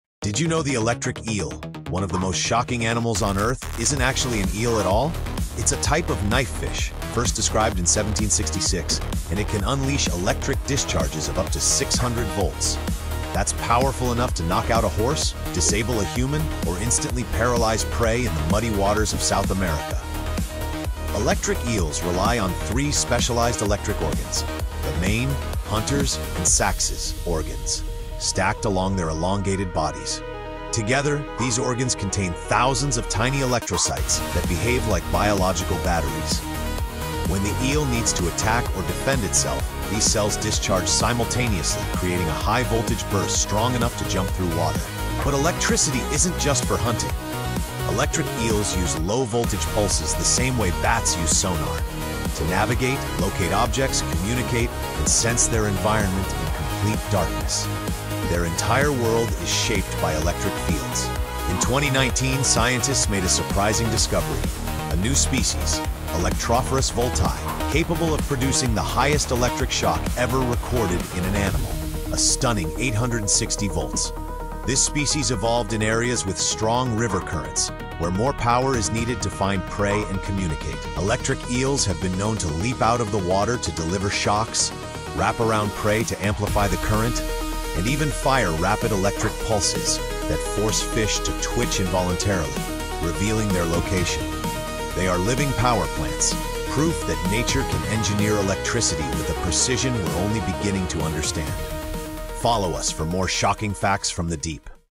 Electric Eel: The Animal That Generates 600 Volts
Electric eels produce one of the strongest shocks in the animal kingdom. Using thousands of specialized cells called electrocytes, they generate up to 600 volts to hunt, communicate, and defend themselves. Their electrical bursts are so powerful they can knock a grown human off their feet.